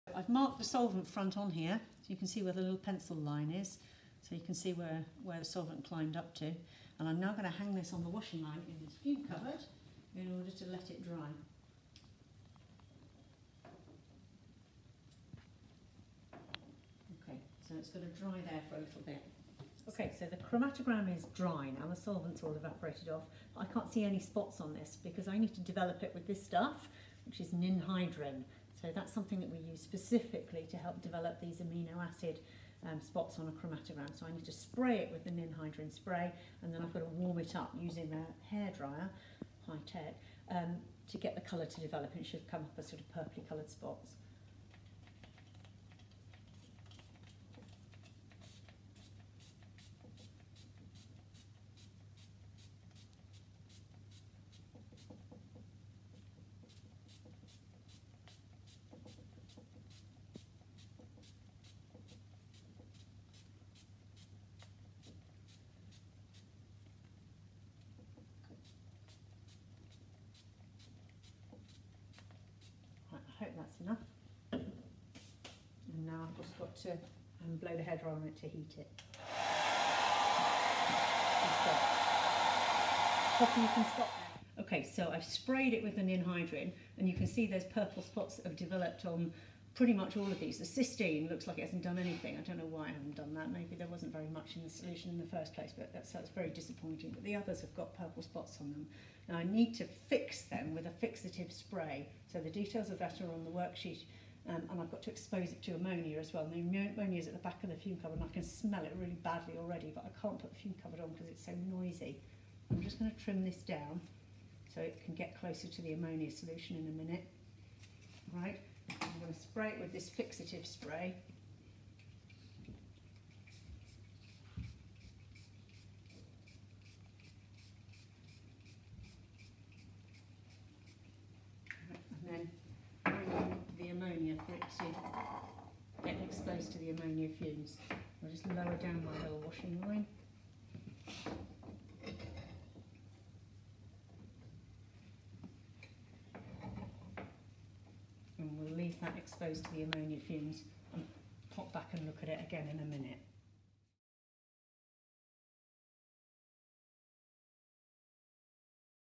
Yr 13 Amino acids chromatography (2)
Developing the chromatogram and calculating Rf